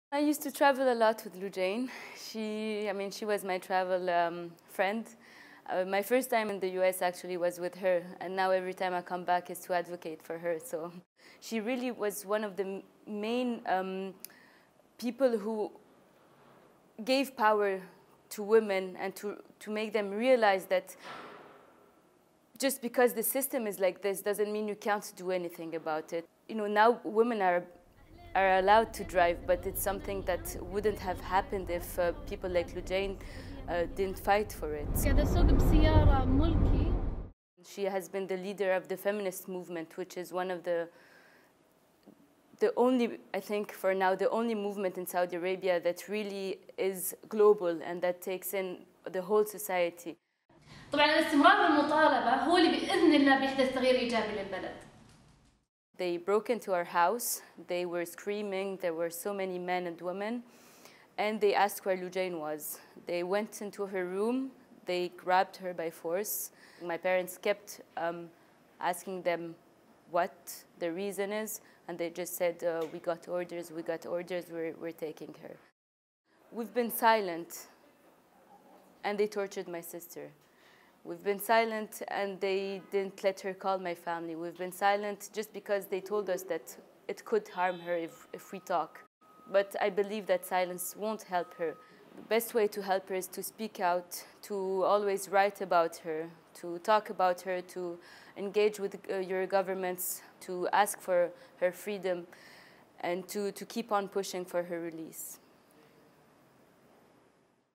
Freedom to Write Index 2019: Lina Al-Hathloul on Saudi writer-activist Loujain Al-Hathloul
Loujain Al-Hathloul is being held in a Saudi prison for her writing and activism around women's rights. In this video, her sister Lina discusses her case and why calling attention to it is the most effective means of calling for her release.